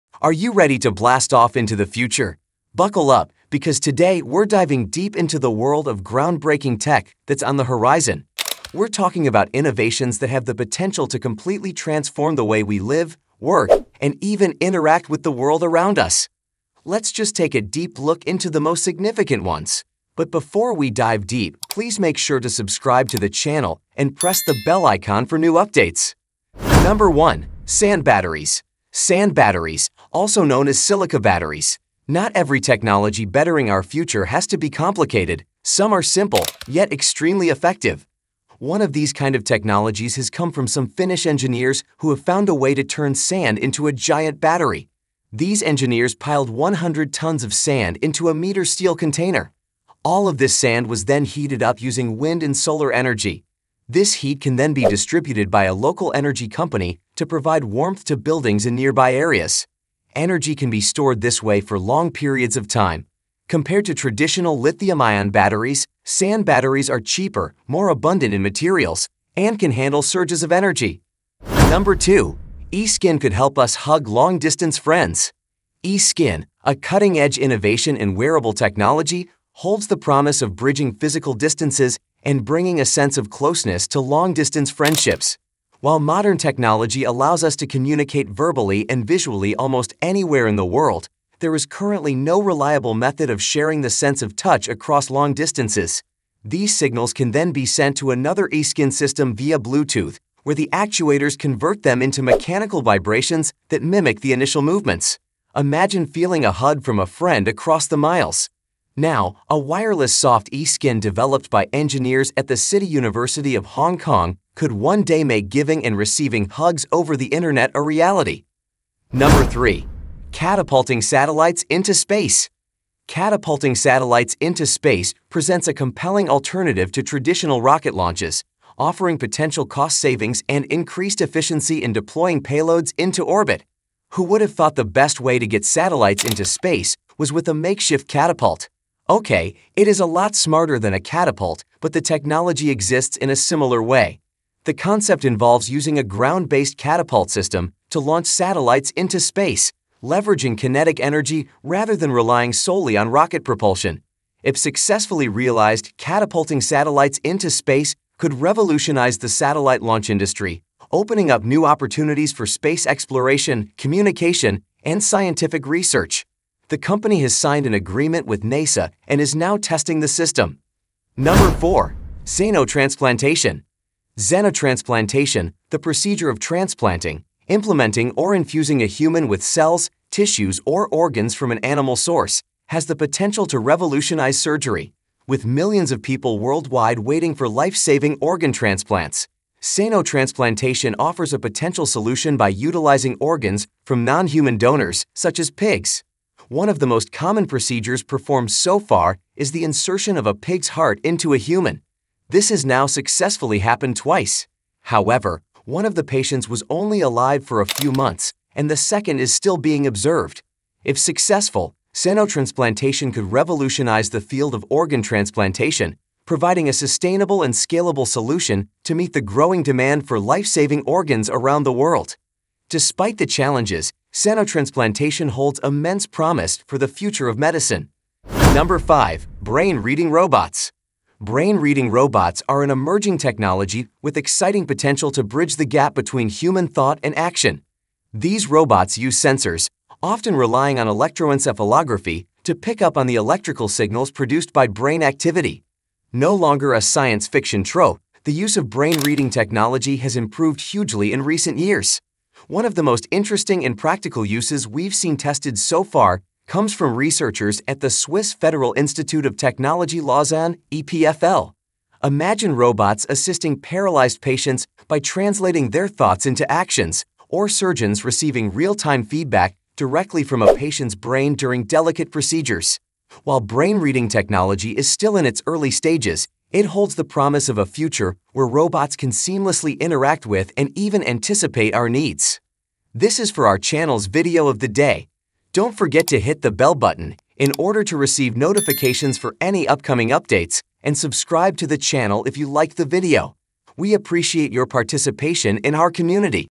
Top 5 technology that will change the future
Curious about the future? Check out the top 10 technology that will change the future in this video! From AI to biotech, the future is looking bright.
In today's video, we delve deep into 5 groundbreaking technologies that have the potential to revolutionize how we live, work, and connect with each other.

Sand Batteries: Discover how Finnish engineers are turning ordinary sand into a massive energy storage solution, providing warmth to buildings and offering a cheaper, more sustainable alternative to traditional lithium-ion batteries.
E-skin: Imagine being able to feel a hug from a long-distance friend! Learn about the latest advancements in wearable technology that could bridge physical distances and bring us closer together through touch.
Satellite Catapults: Forget traditional rocket launches! Explore the concept of using ground-based catapult systems to propel satellites into space, potentially revolutionizing the satellite launch industry and opening up new opportunities for space exploration.
Xeno-transplantation: Explore the future of organ transplantation with xenotransplantation, a procedure that utilizes organs from non-human donors, such as pigs, to meet the growing demand for life-saving organs worldwide.
Brain-Reading Robots: Delve into the realm of brain-reading technology, where robots can interpret human thoughts and translate them into actions, offering incredible possibilities for assisting paralyzed patients and enhancing surgical procedures.
Join us as we journey through these incredible advancements that are reshaping our future! Your support means the world to us as we continue to explore the frontiers of innovation together.